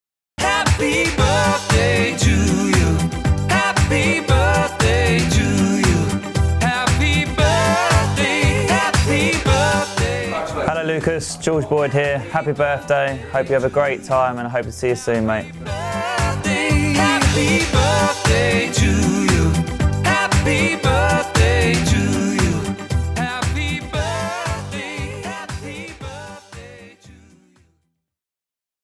Happy birthday Lucas Hardy
A very special birthday wish for Junior Clarets member Lucas Hardy from George Boyd!